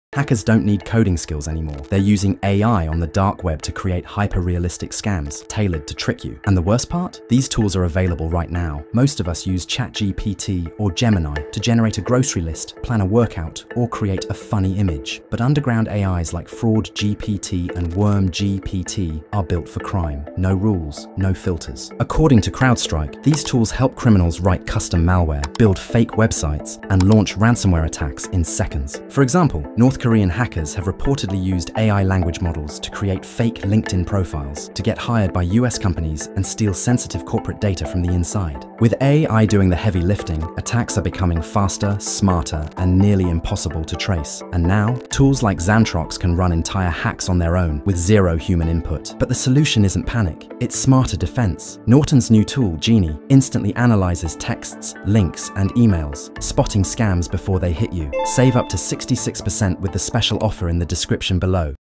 How AI Is Helping Dark Web Hackers Grow
AI is changing everything—including cybercrime.
Hackers are now using dark web AI tools like FraudGPT and WormGPT to launch powerful malware attacks, phishing scams, and ransomware with zero coding skills.

Cyber attacks are becoming faster, smarter, and harder to stop.

*By making a purchase through this link you're supporting the channel. This comes at NO cost to you. Thanks.